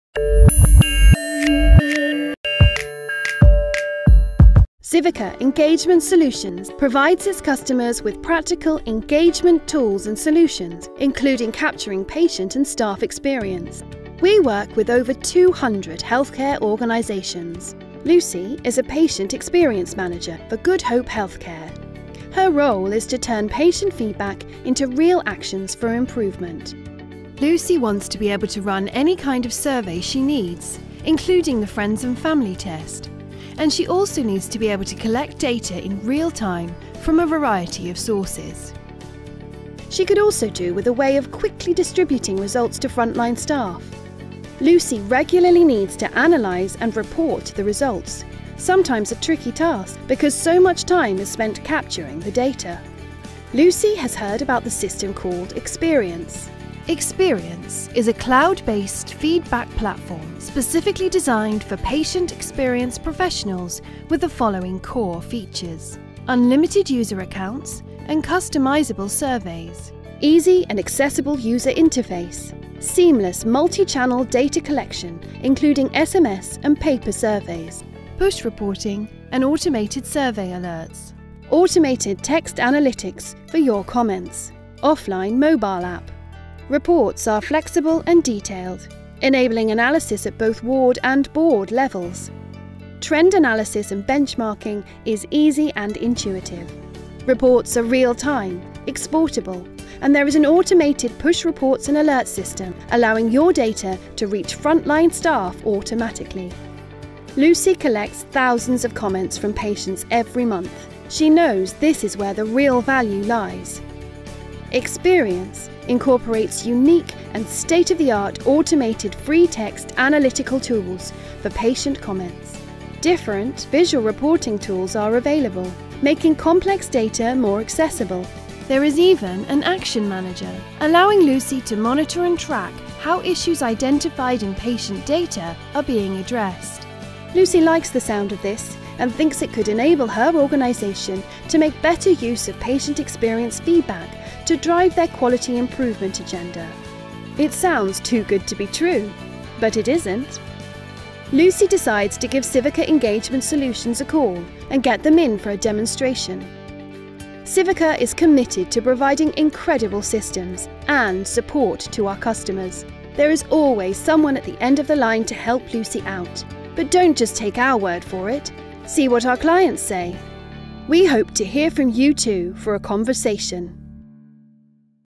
Civica Experience: Harnessing patient feedback to improve care
Civica Experience enables healthcare organisations to understand real-time patient feedback, prioritise risk areas and drive service improvement, as well as highlighting positive comments. Our software provides multi-channel data collection, bespoke analysis and reporting.
Every day Civica analyses thousands of patient experiences for healthcare organisations in the UK and Australia, including the NHS Friends and Family Test, providing a continuous feedback loop between the patient and healthcare professional.